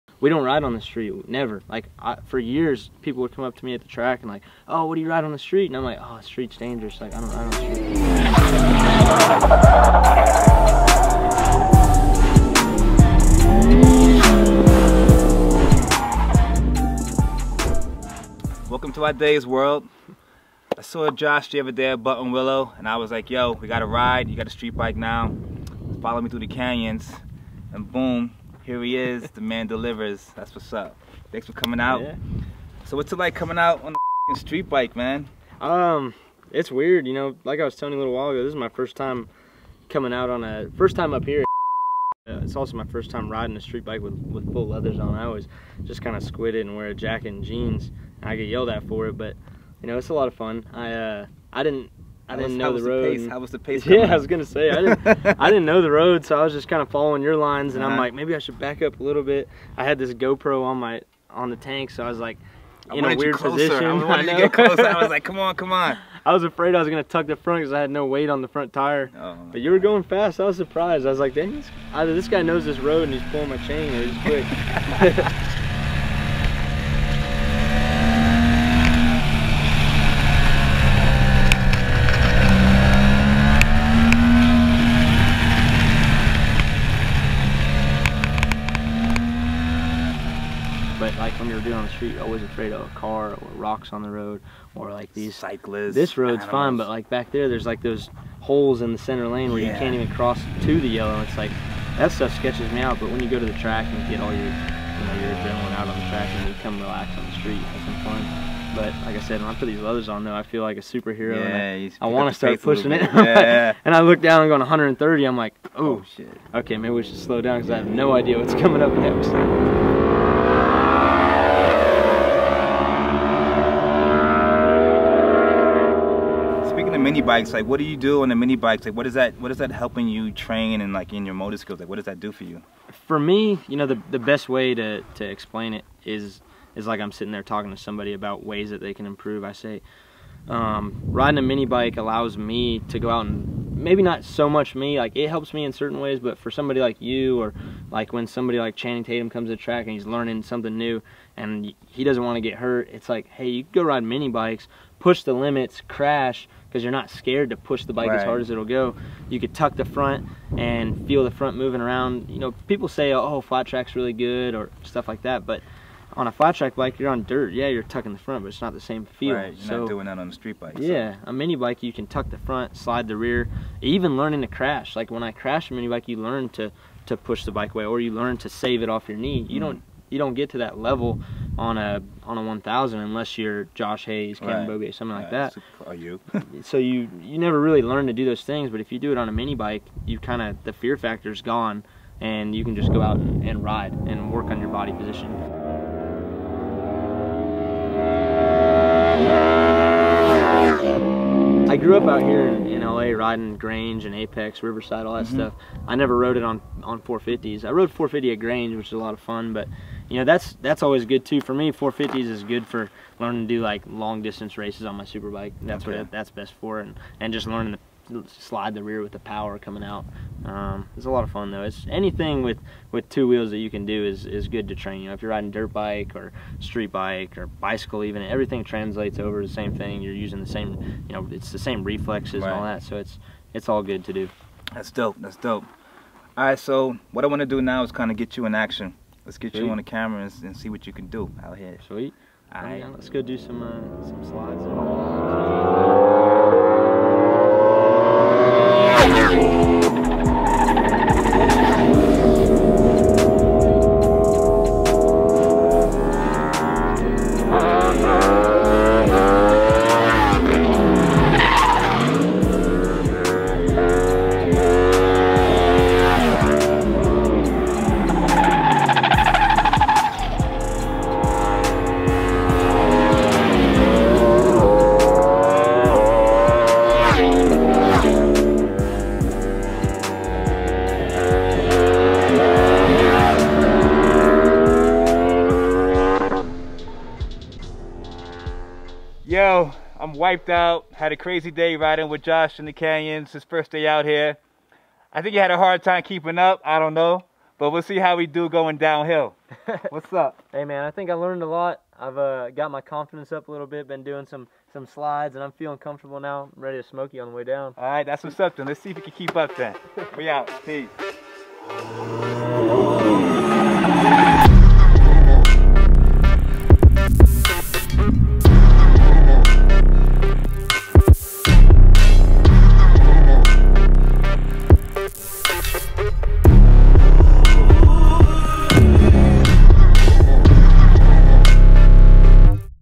Riding with Tire Slayer Josh Herrin
Sat down for a little interview with the great Josh Herrin. The skills this dude has on a bike are UNREAL!